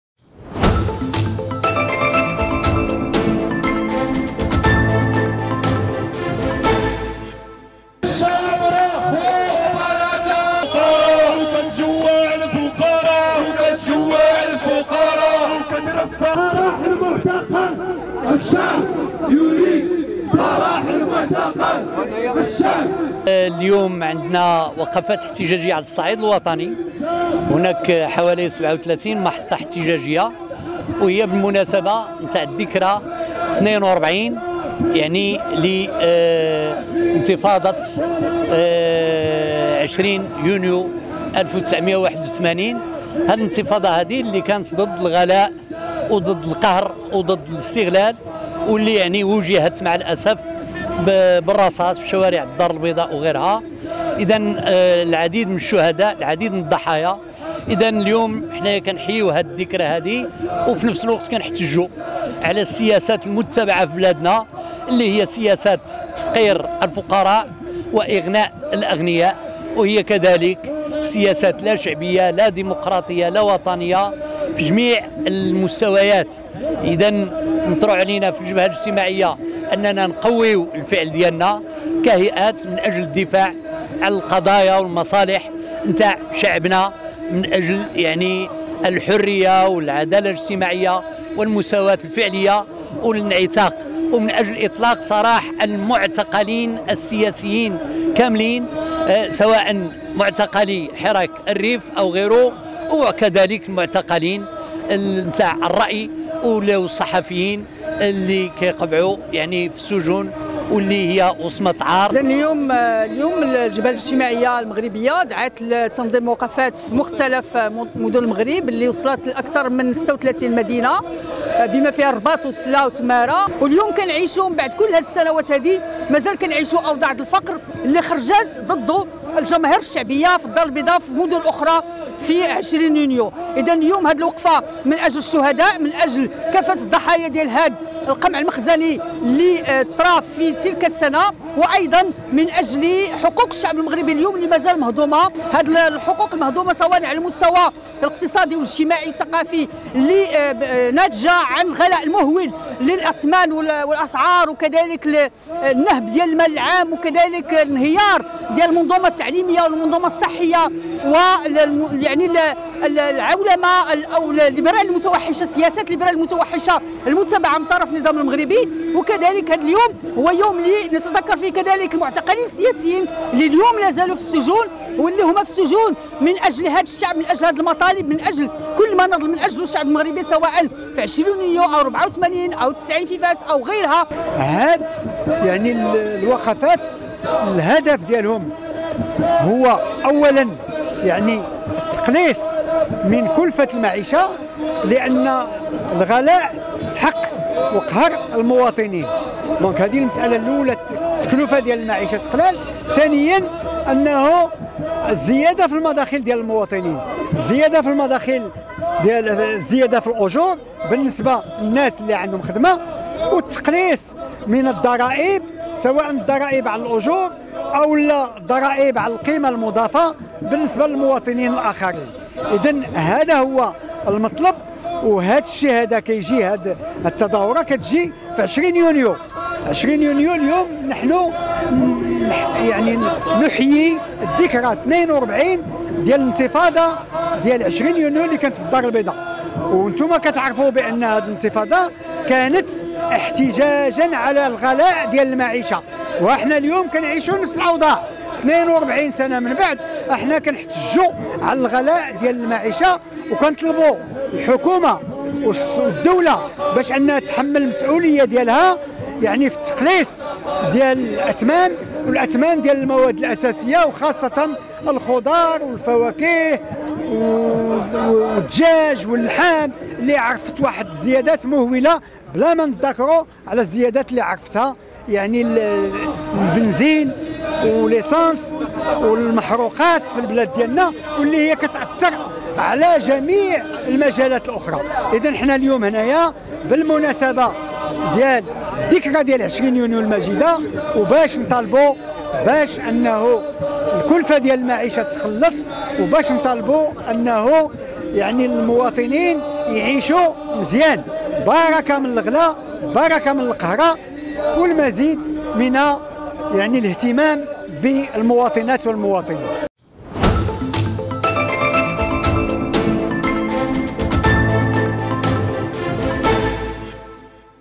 بالرباط..وقفة احتجاجية ضد الغلاء في ذكرى انتفاضة 20 يونيو 1981
ظمت الجبهة الاجتماعية المغربية اليوم الثلاثاء 20 يونيو 2023 وقفة احتجاجية أمام البرلمان "بمناسبة الذكرى الـ42 لانتفاضة 20 يونيو " بالدار البيضاء